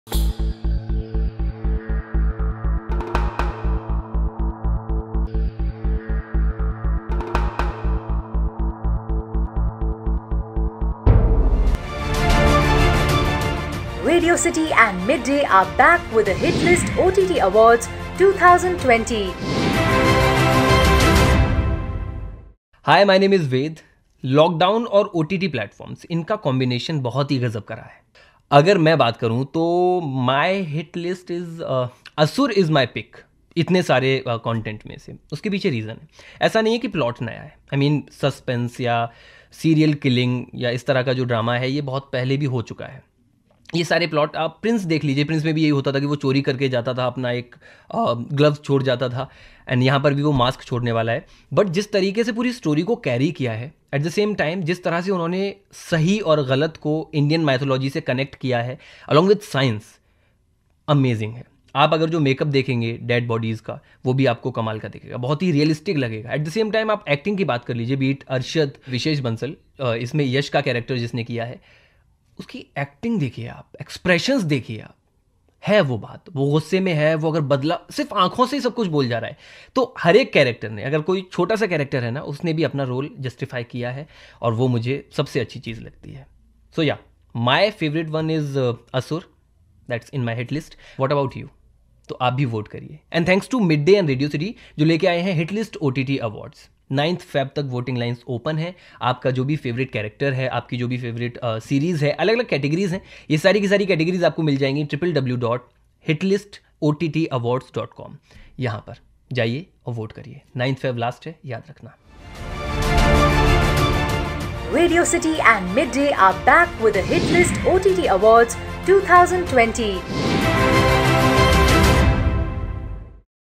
Asur: RJ Ved picks his favourite web show from 2020 on My Hitlist | Hitlist OTT Awards
Jaipur's RJ Ved shares what makes Voot Select's 'Asur' his favourite web show from the year 2020, only on MyHitlist.